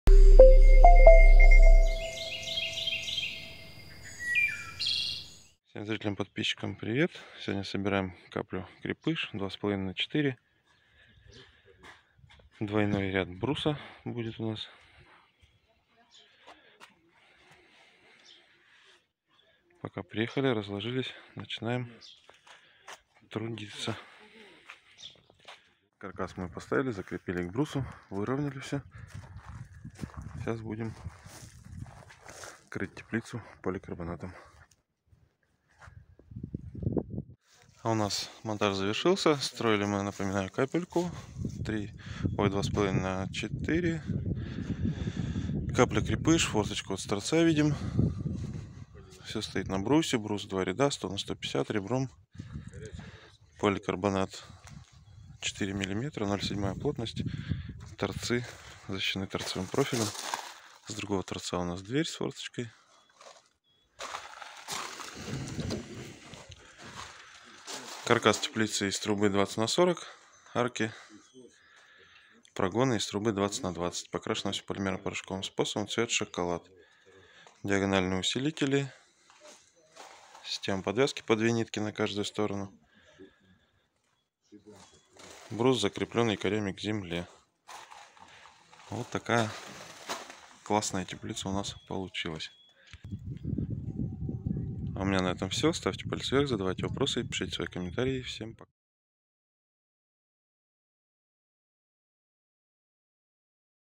КАПЕЛЬКА 2,5М КРЕПЫШ. ШИРИНА ТЕПЛИЦЫ 2,5 МЕТРА.
Теплица которая выращивает все сама?
Фантастика?
НЕТ!

С вами Компания Уральская Усадьба 🌱🌱🌱

теплица «Капелька 2,5М»
Начнем с фундамента:
   Брус 100х150 в два ряда, зачем?
1) Теплица выше
2) Фундамент высотой 300мм уже является основой для грядок, остается только обшить плоским асбесто листом и сделать короб и засыпать землю
3) Брус пропитан специальным антисептиком в камере на производстве!
    Проветривание
В этой теплице автоматически открывается все! Форточки торцевые огромного размера 800х880мм! Также можно открыть дверь, ее размер просто огромный! 1800х890мм.
    Подвязывать растения? Пожалуйста! Четыре троса ¾ в пвх помогут вам в подвязке урожая!

Покрытие: самый лучший поликарбонат на российском рынке, Новатро 4мм плотность 0,75 кг/м2 и соты защищены перфолентой и торцевым профилем.

Каркас изготовлен из профильной оцинкованной трубы двух видов 20х40 и 20х20.
Гальваническое цинкование и полимерно порошковая покраска, обеспечивает 100% гарантию от ржавчины!
Цвет каркаса 8017 шоколад, шикарный цвет! так и просится на участок!

И все вместе - это двойная защита от коррозии и ржавчины!
Снежная зима??
Нет возможности чистить снег??
Купи теплицу  с гарантией 10 лет!
 По снеговой нагрузке без ограничений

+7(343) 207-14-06
🏡 Екатеринбург ул Климовская 23
🖥 ural-usadba.ru